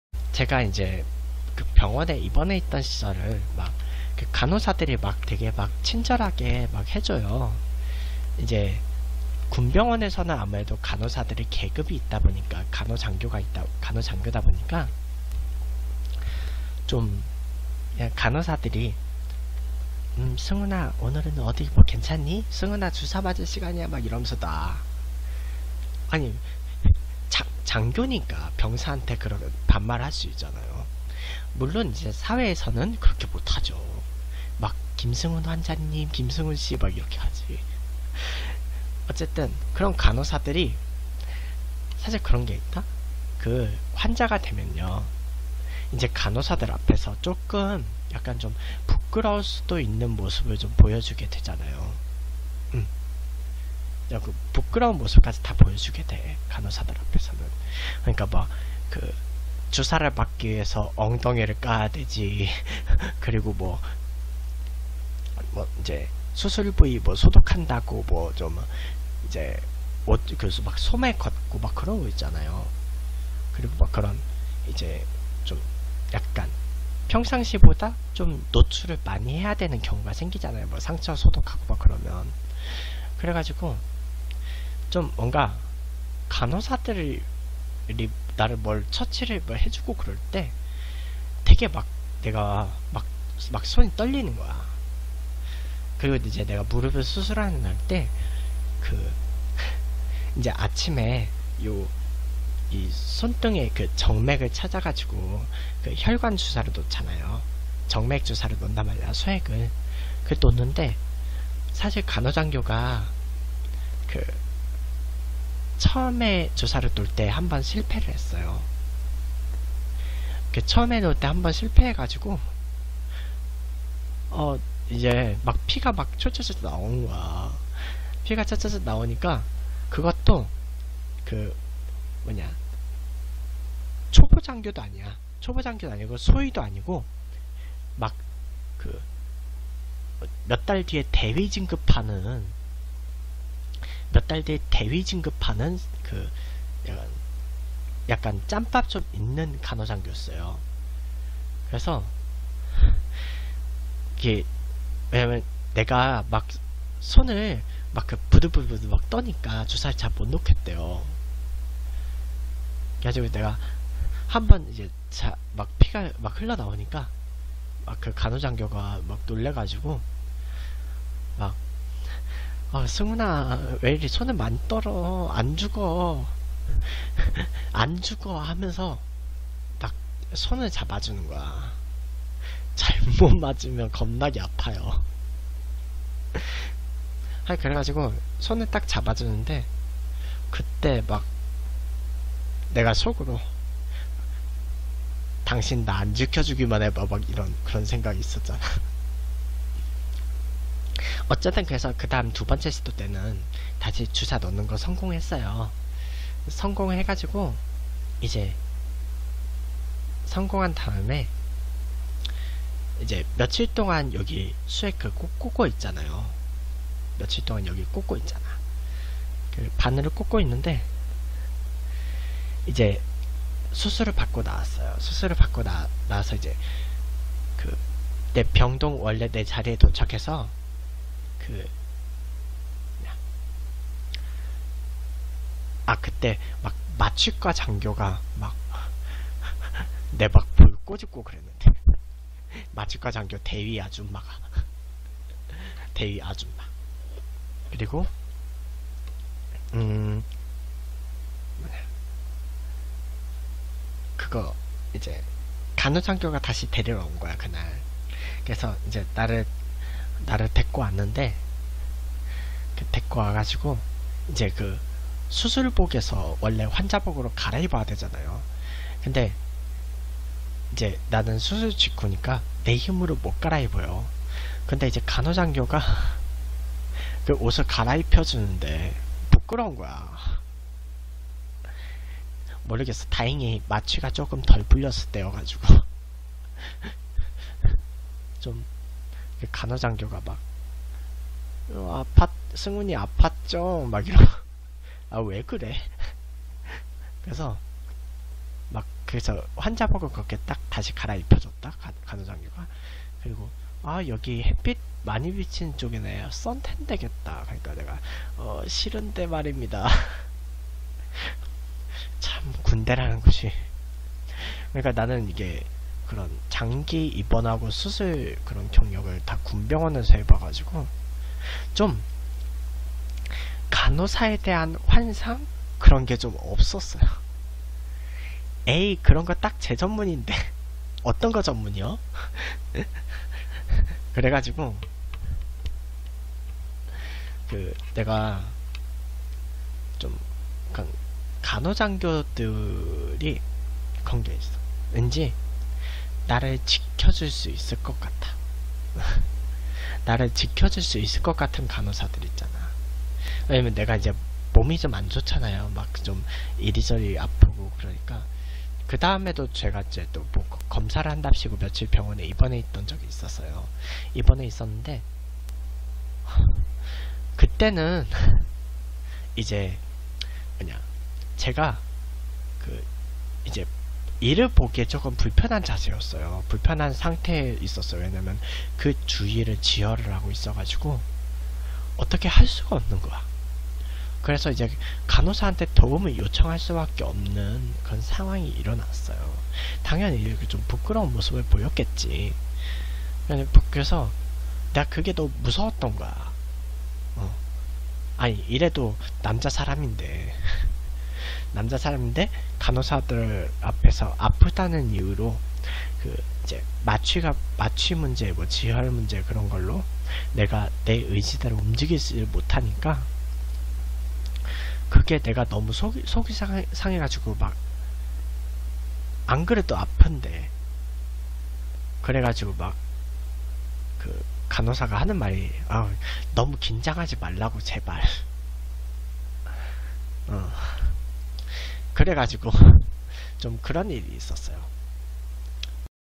간호사들과 얽힌 추억
병원에 길게 입원해 본 적이 있었던 지식테이너.
그랬던 만큼 간호사들과의 추억이 가끔 떠오른다는데...